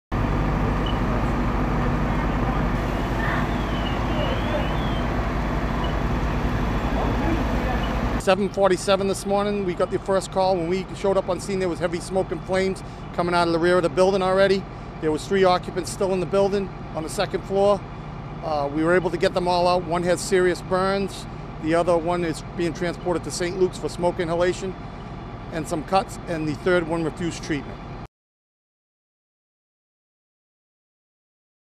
VIDEO NOW: Two injured in a Fairhaven fire
Two people injured after a fire at Jackson's Variety in Fairhaven.